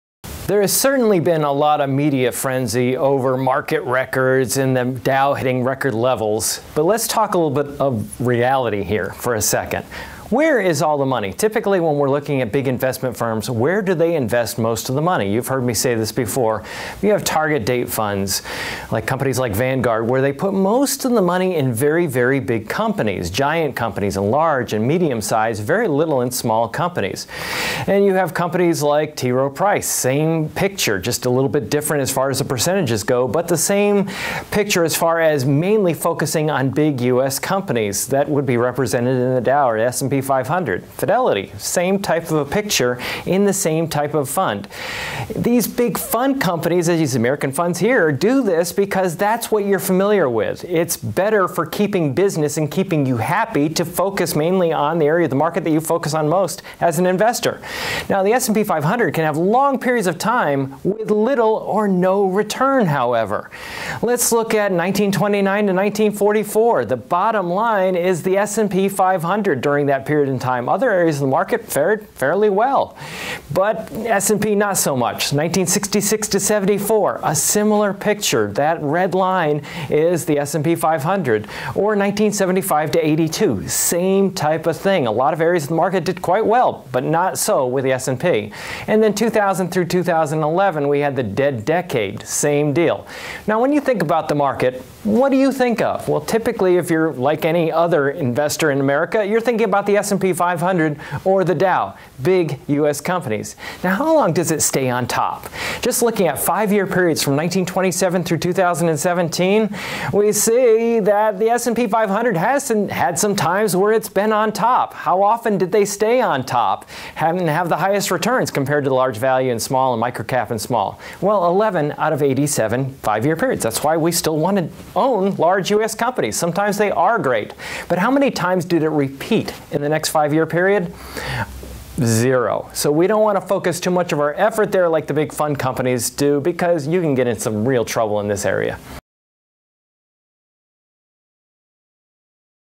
Market Records and Reality
The media is obsessed with how hot the market has been; is it overvalued? Is it going down soon? In this video I explain what we should really be concerned about.